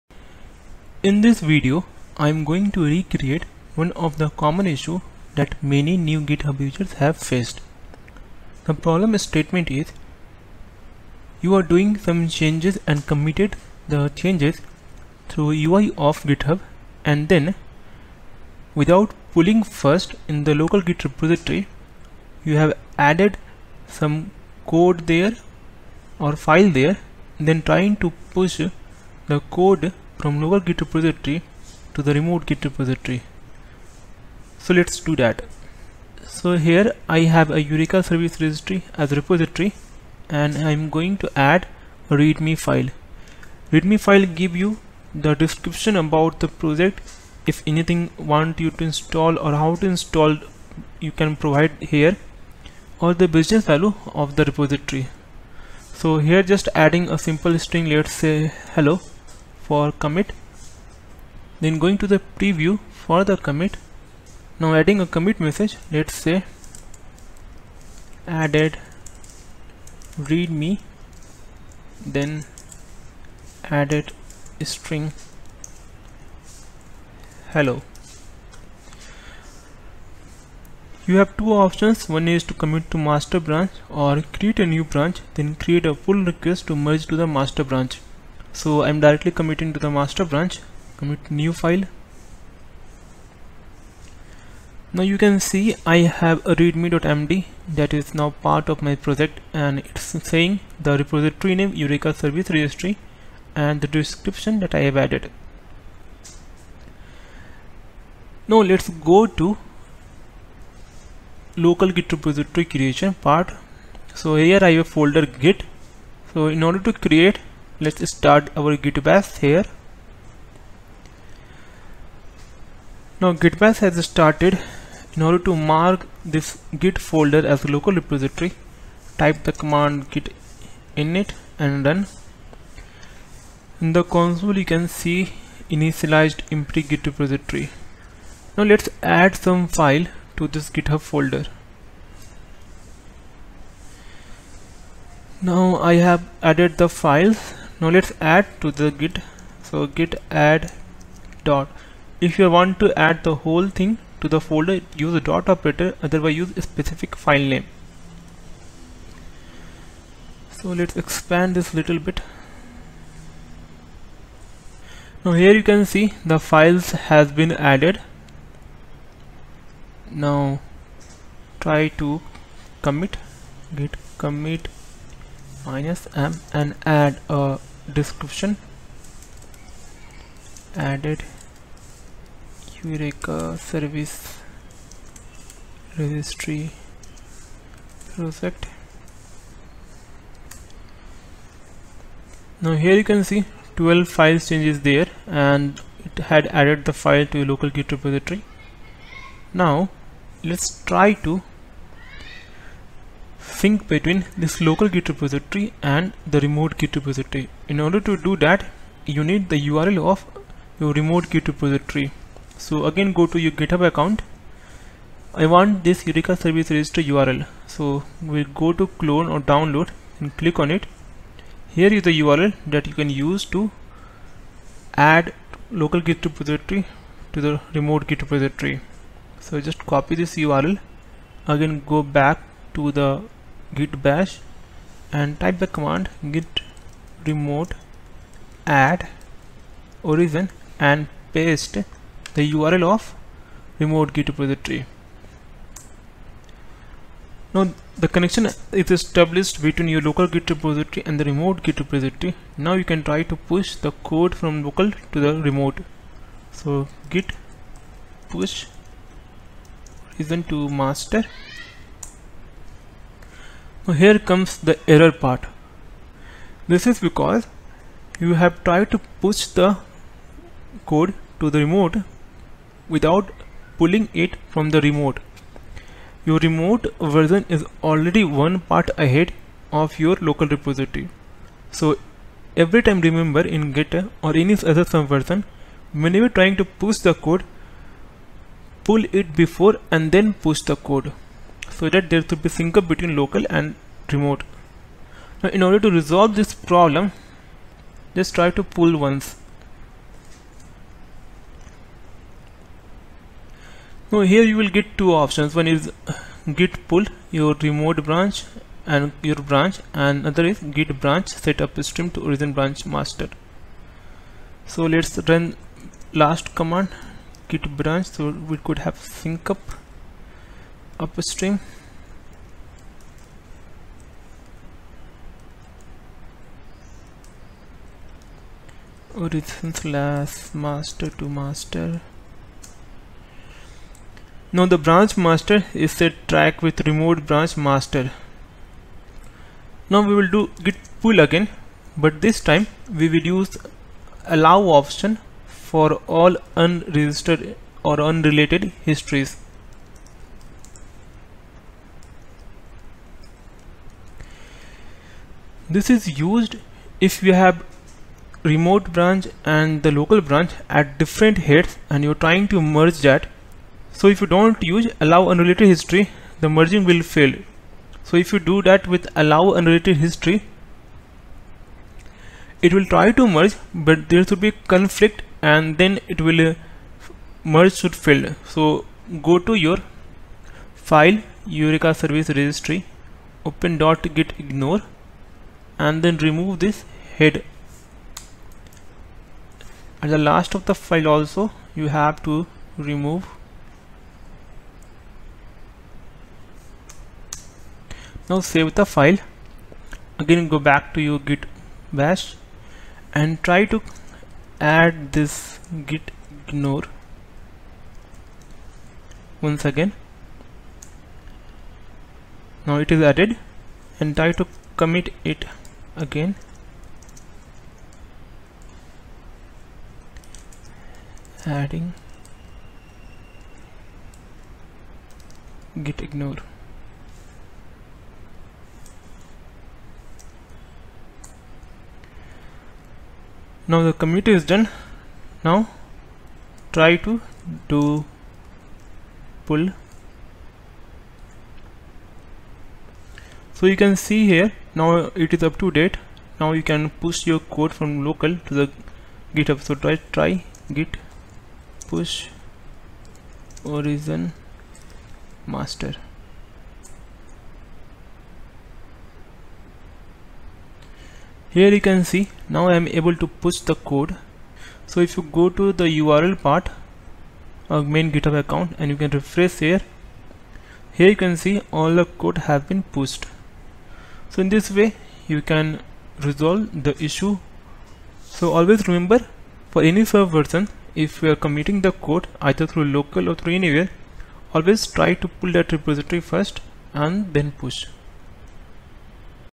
GIT : Resolving " Code push Rejected "
GIT is most used code repository.
Problem Statement :
Sometimes, we forget to pull before Push. In such scenario, if commit has been done on remote git repository already and file changes is done in local and committed before pulling from remote one , then code  push to remote repository will fail.
'Solution :
Perform following steps:
   1.   git pull --allow-unrelated-histories
   2.   Manually remove comment in .gitignore (if any) done after pull
   3.   git commit -m "Commit message"
   4.   git push source_branch destination_branch

So, Always remember to do PULL from the repository before PUSH code to remote repository.